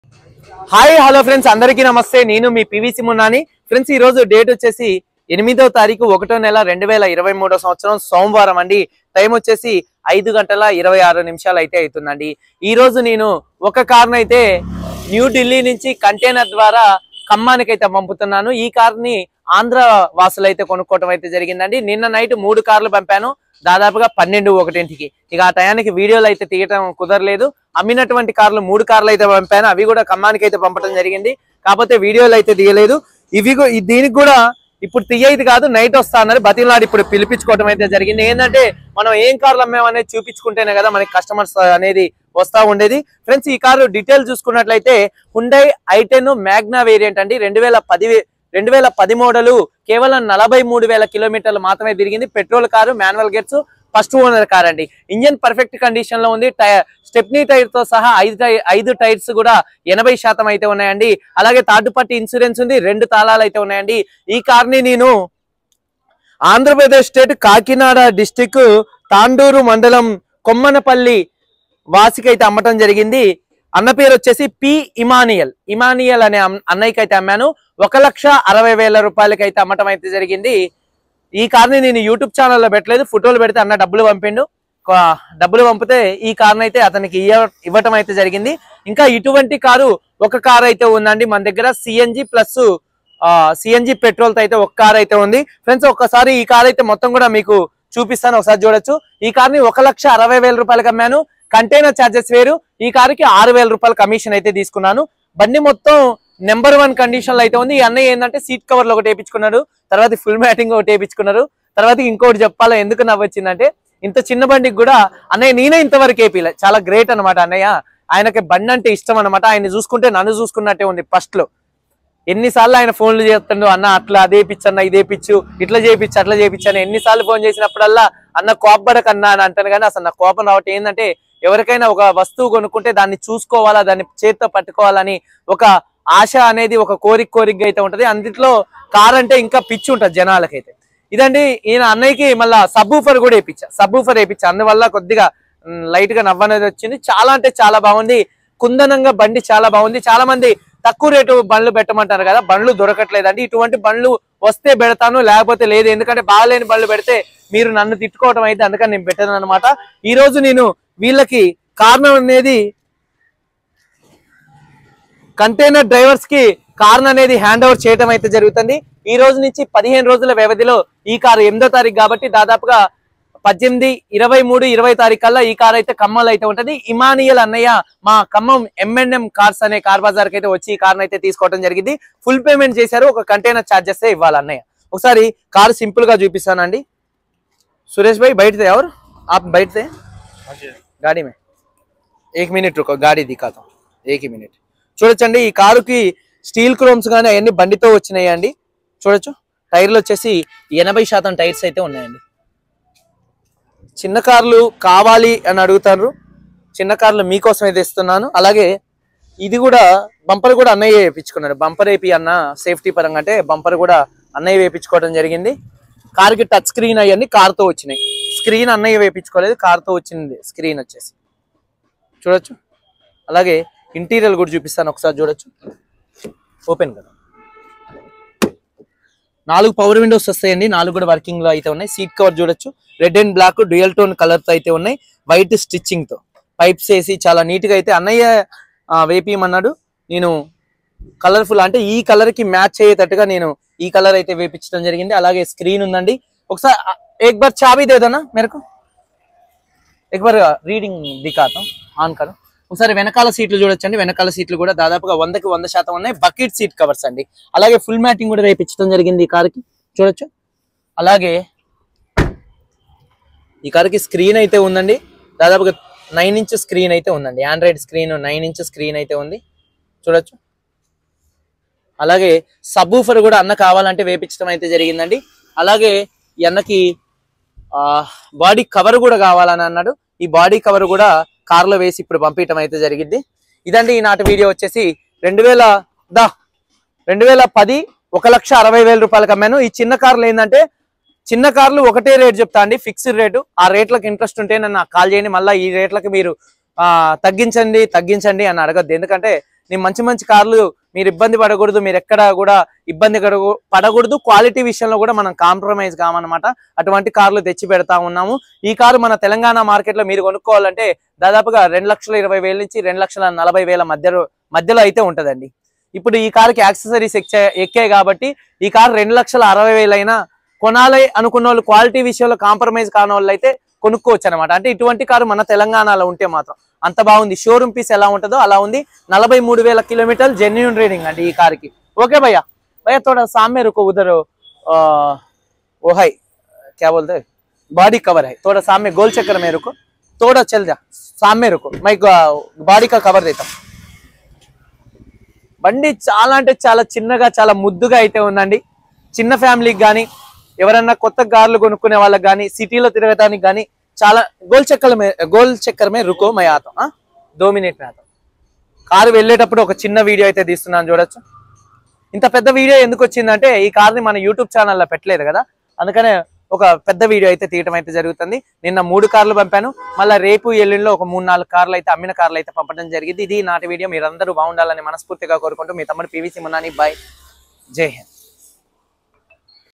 ఆంధ్రప్రదేశ్ వాసులకు న్యూఢిల్లీలో i10 Magna 2010 ఇప్పించడం జరిగింది కంటైనర్ ద్వారా పంపుతున్నాను💐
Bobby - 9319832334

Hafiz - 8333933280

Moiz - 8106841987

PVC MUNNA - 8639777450